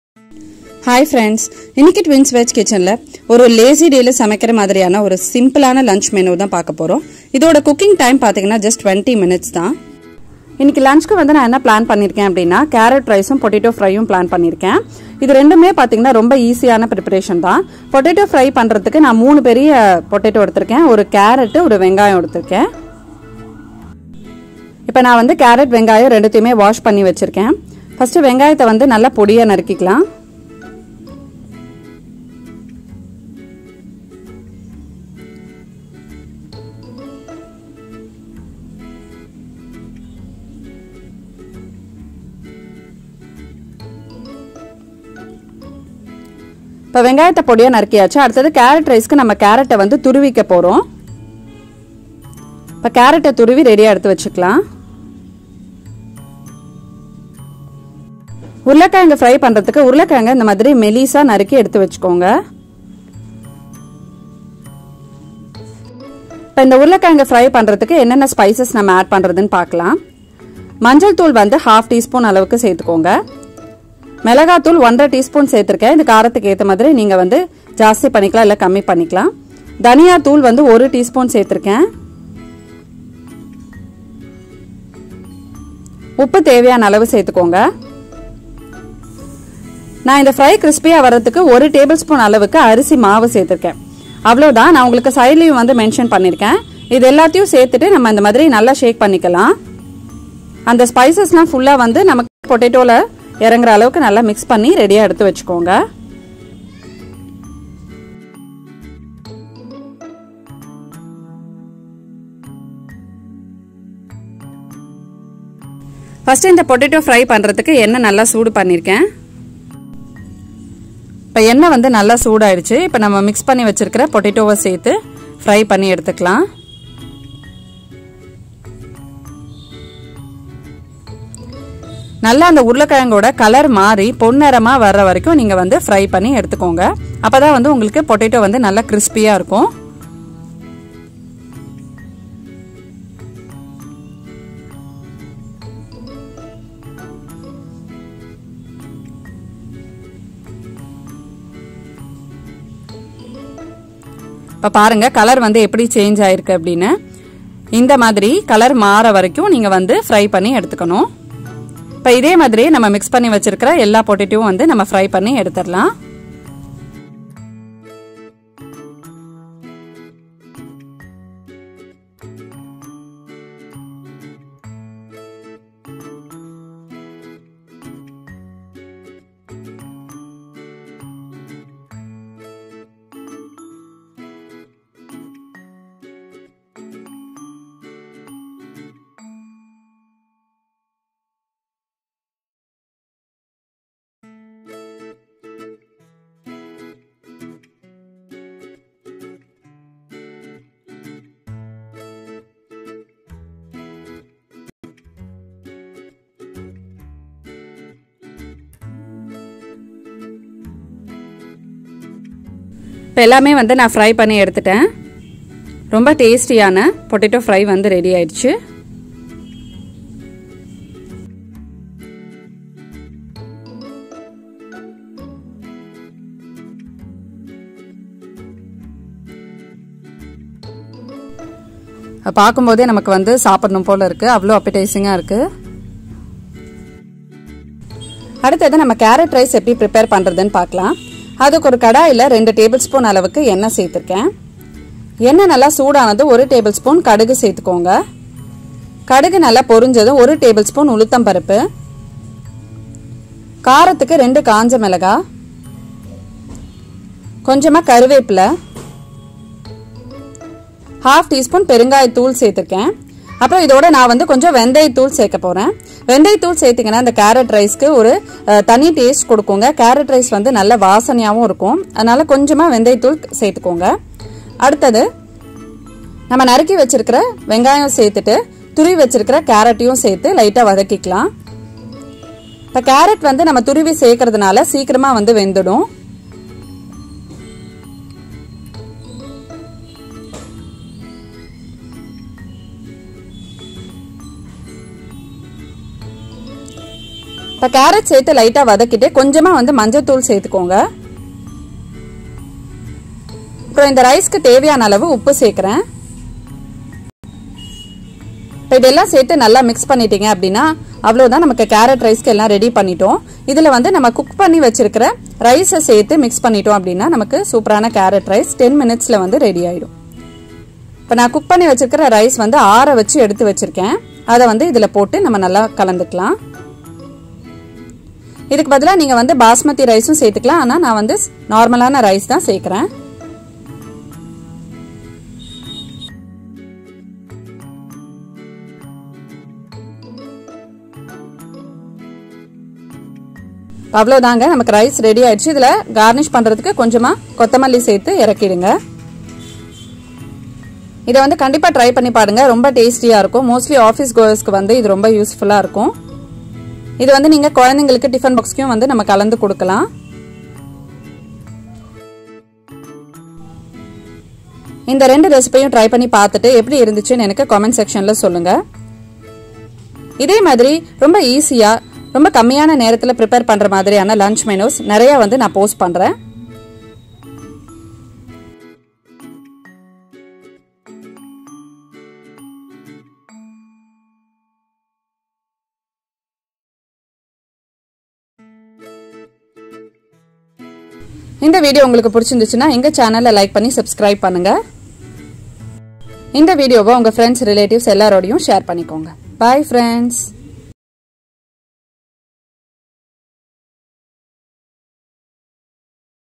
Lazy day easy Samayal || Carrot rice || Potato fry || Prepare in just 20 minutes || Lunch ideas
Hai friends , this video will be very useful for office goers .